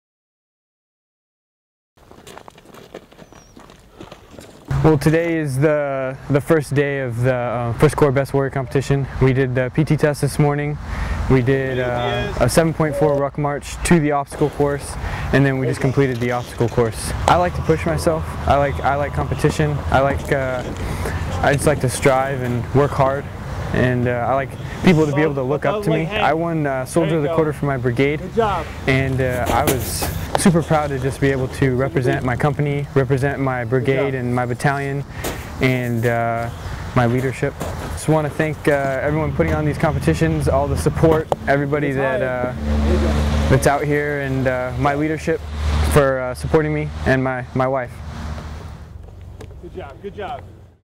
I Corps holds Best Warrior Competition
Every Quarter, I Corps holds the Best Warrior Competition on Joint Base Lewis, McChord, Washington. Specialist Joshua Diaz tells us why he chooses to compete in this grueling, but rewarding event.